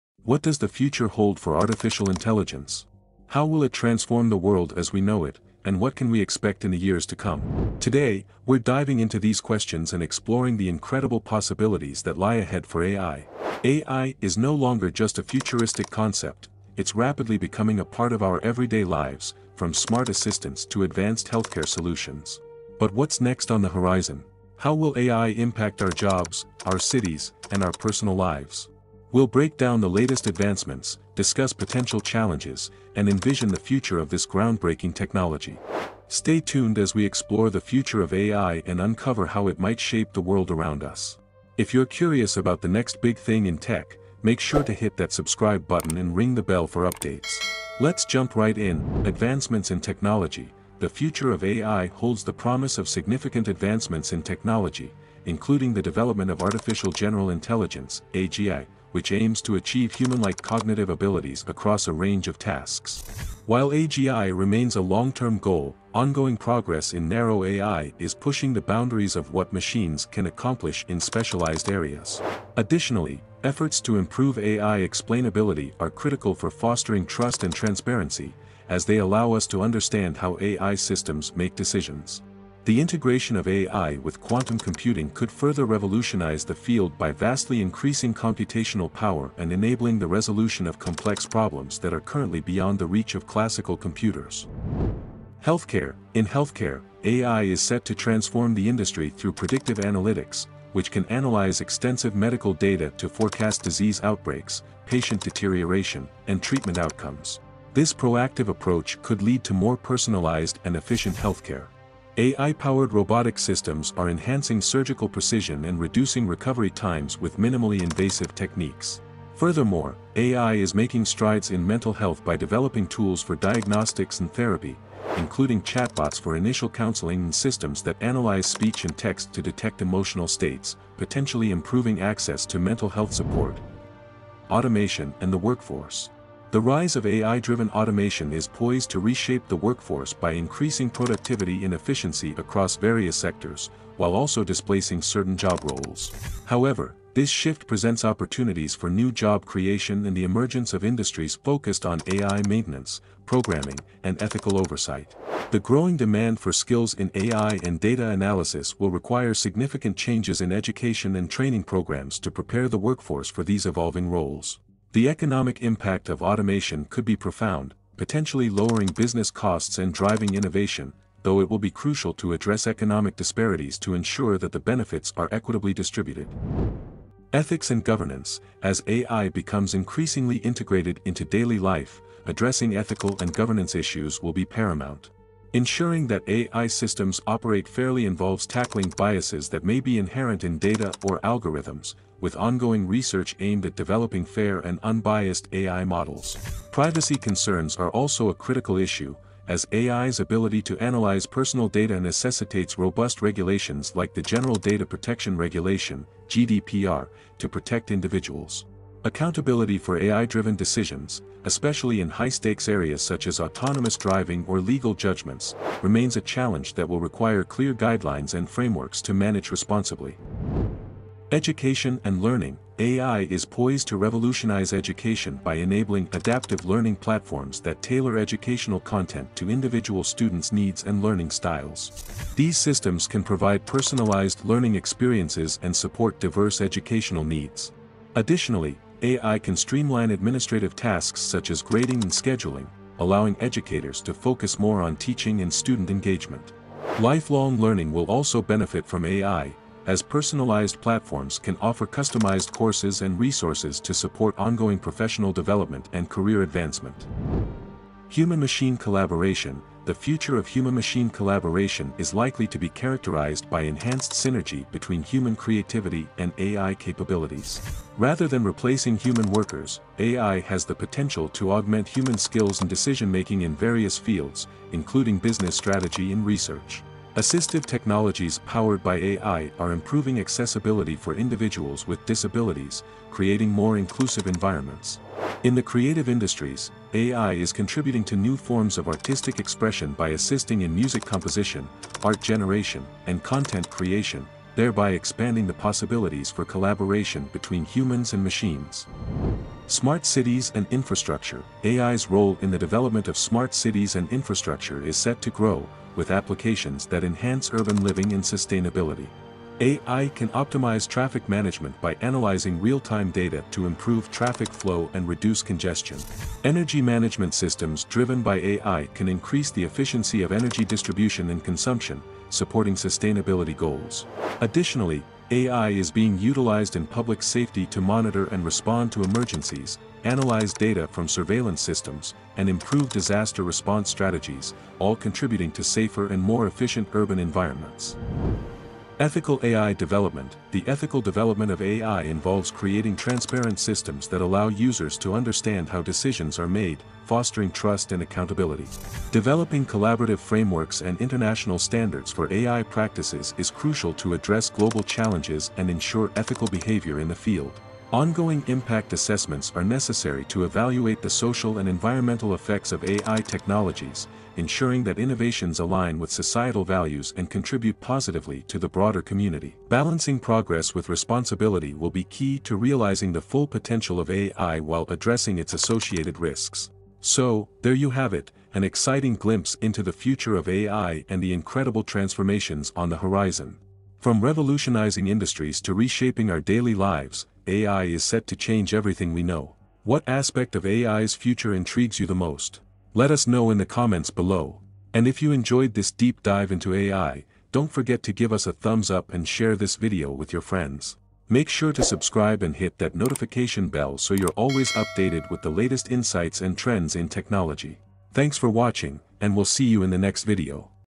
The Shocking Truth About AI's Future! You Won't Believe What's Coming Next!
In this video, we explore the captivating future of artificial intelligence (AI) 🤖 and its potential to transform various aspects of our lives 🌍. We discuss the latest advancements in AI technology, from breakthroughs in machine learning 🧠 and neural networks to emerging trends in AI research and development 🚀. Discover how AI is reshaping industries such as healthcare 🏥, finance 💰, transportation 🚗, and entertainment 🎮, and delve into the ethical considerations ⚖️ and challenges associated with AI's rapid growth.

We'll also explore how AI could impact job markets 👨‍💻, everyday experiences 🏠, and global economies 🌐, offering expert opinions and predictions on what to expect in the coming years 📅. Whether you're a tech enthusiast 💡, a professional in the AI field 📊, or simply curious about the future of technology 🔮, this video provides comprehensive insights and thoughtful analysis on what lies ahead for artificial intelligence. Join us as we navigate the exciting future of AI and its potential to revolutionize our world 🌟.